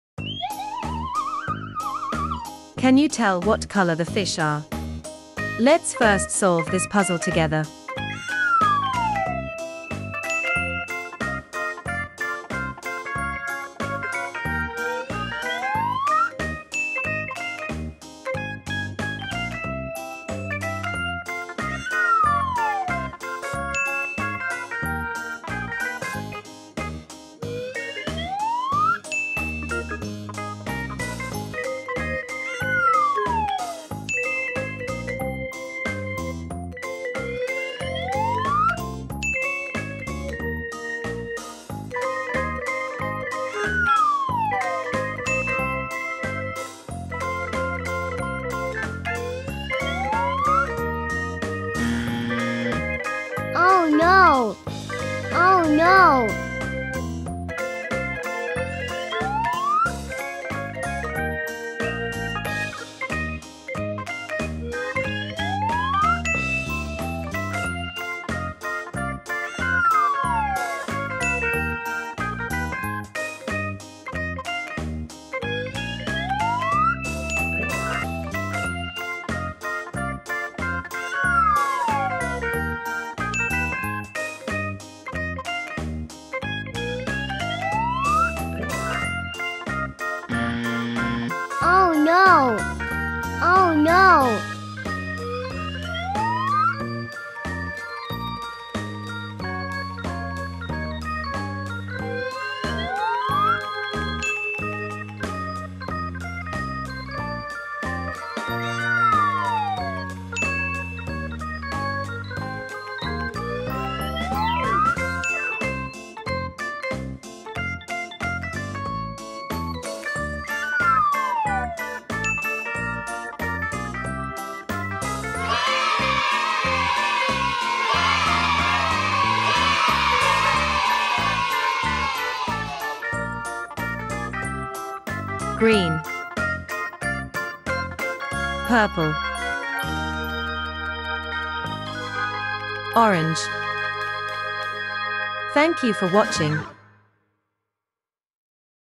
Secondary colors || Purple, Green and Orange || Jigsaw puzzle
Learn secondary colors using a jigsaw puzzle